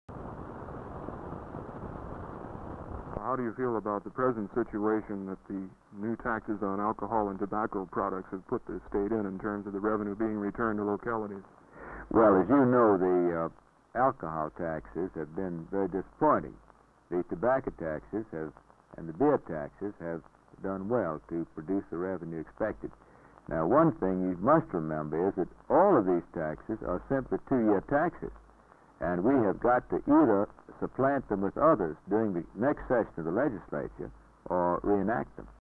04/12/61 State Senator Boothe discusses term limits for speaker of General Assembly
6168
State Senator Armistead L. Boothe of Alexandria says its essential to vital, growing Democratic Government to have a periodic turnover in the Speakership of the Virginia House of Delegates.
Speaking in Roanoke last night, Boothe said he was not talking about any individual. But that he would like to see a return to the old policy under which the speaker served 3 terms and was replaced.
The Present speaker --E. Blackburn Moore of Berryville--has held the post since 1952--longer than any man in modern times.
Boothe said the most vital need of the state is education.
(Start Film)
He discussed a number of other topics, including the states alcoholic tax and tax on tobacco.
(show film)
(((Sound Up: SOF)))